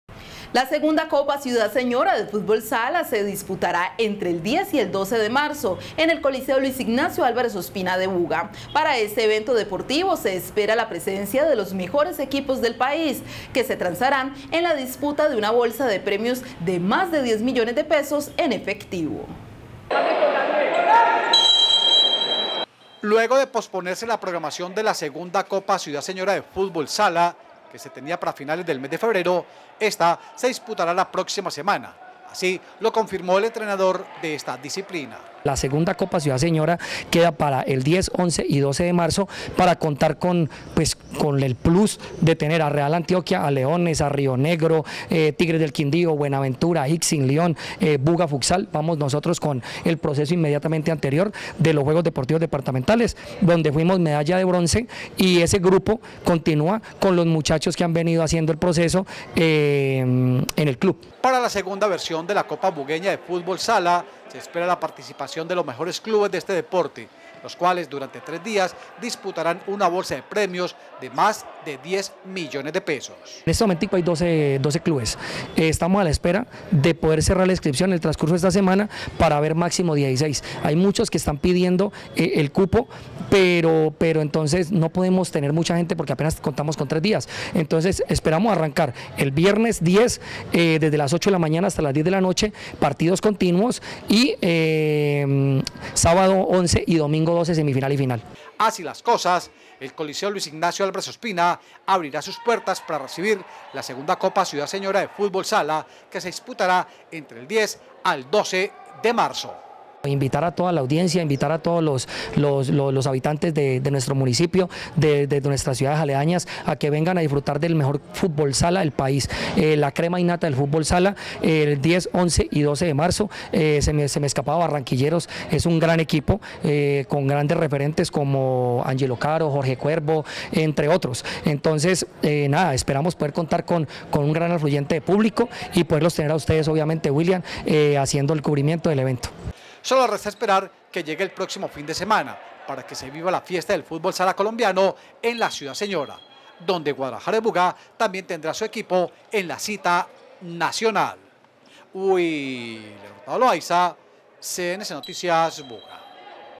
2da Copa Ciudad Señora de Fútbol Sala,
La 2da. Copa Ciudad Señora de Fútbol Sala, se disputará entre el 10 al 12 de marzo, en el coliseo Luis Ignacio Álvarez Ospina; para este evento deportivo se espera la presencia de los mejores equipos del país, que se trenzarán en la disputa de una bolsa de premios de más de 10 millones de pesos en efectivo.